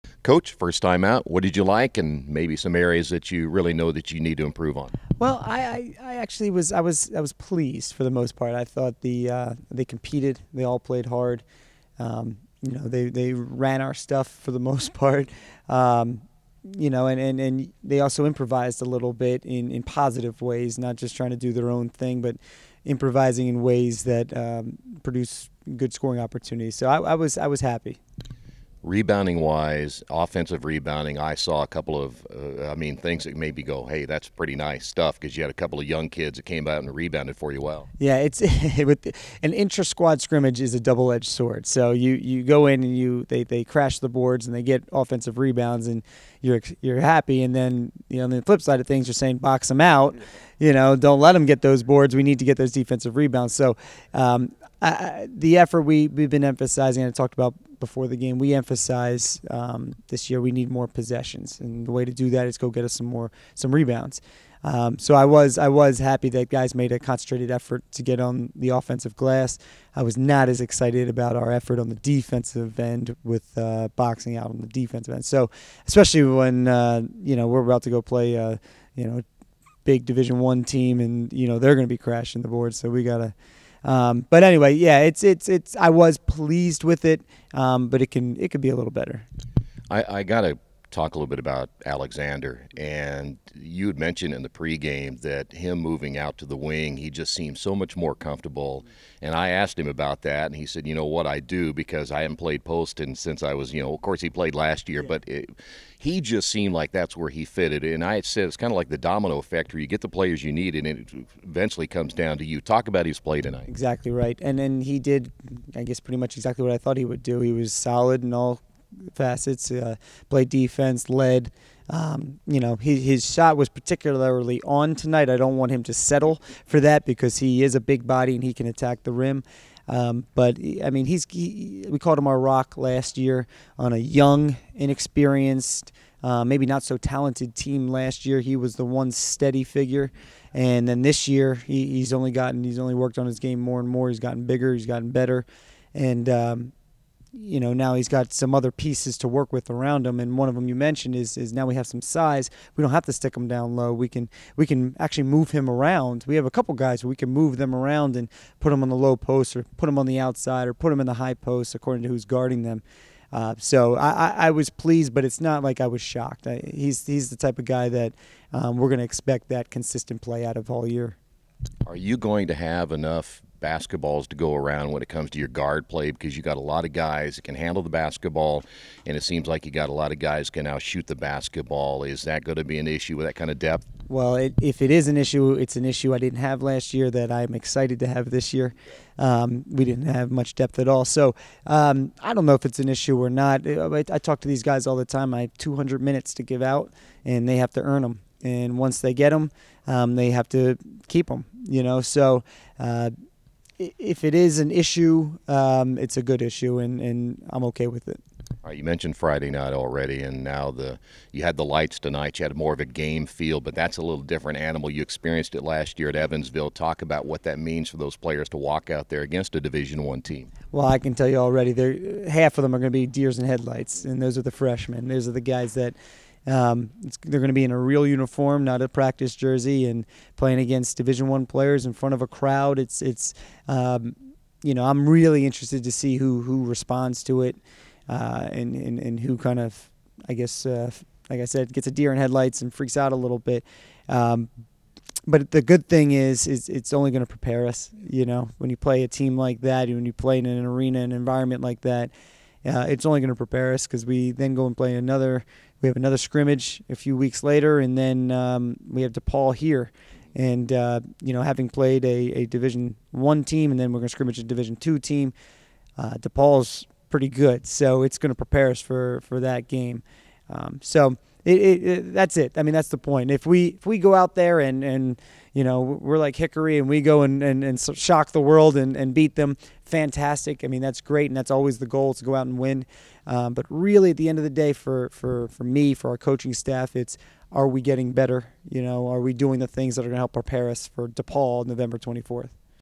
JASON POLYKOFF
COACH POLYKOFF TALKS ABOUT THE FIRST SCRIMMAGE FOR EARLHAM